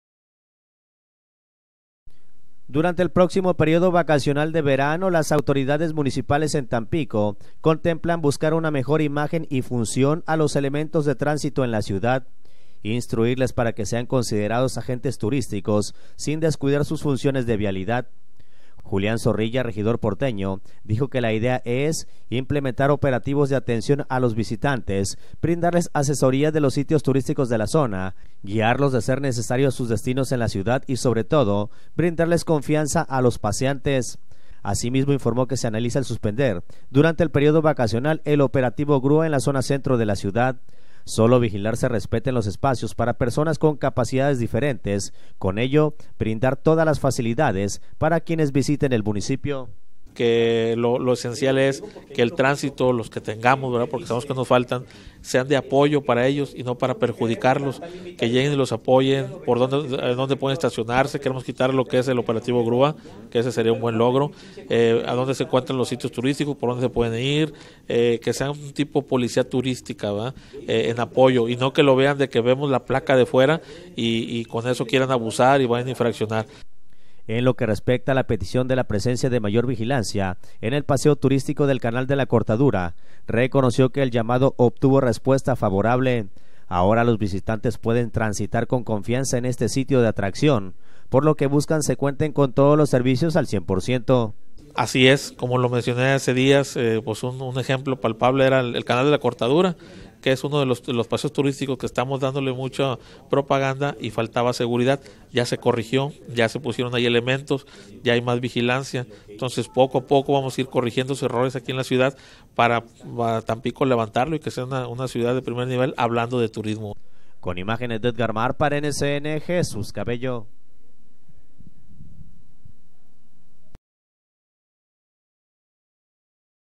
TAMPICO NCN IMAGEN TRANSITOS
Se buscara que Durante el periodo vacacional, los elementos de transito en Tampico, tengan una buena imagen y brinden asesoría a los paseantes.